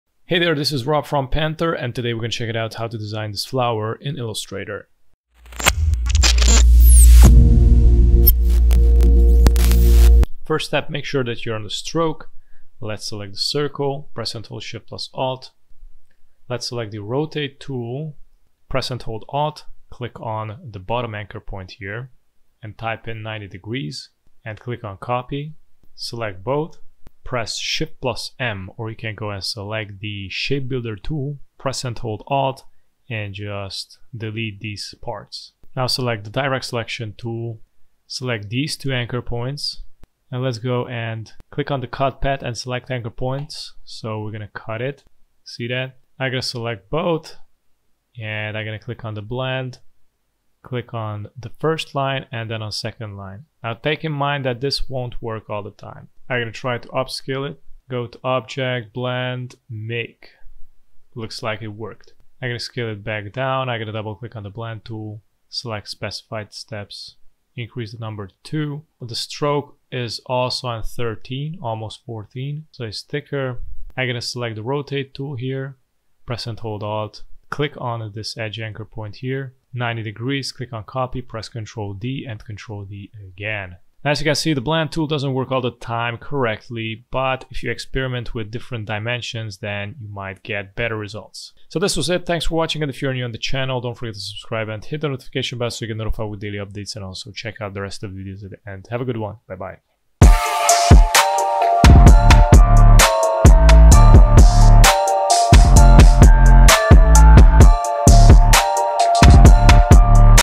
How To Blend Flower In Illustrator
I did this tutorial and decided to leave in the video the failed part where basically the blend tool wasn't working correctly. There are more ways to correct this problems and this is a new one I discovered today.